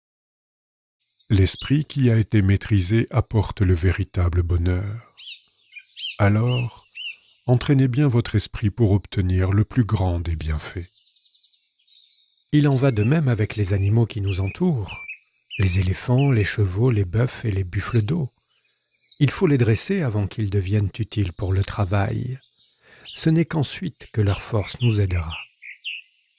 L'esprit Maîtrisé 🪷The Mastered Spirit
The mind that has been mastered brings true happiness, so train your mind well to get the greatest
benefit, it is the same with the animals that surround us elephants
horses, oxen and water buffaloes they must be trained before they become useful for work only then will their strength help us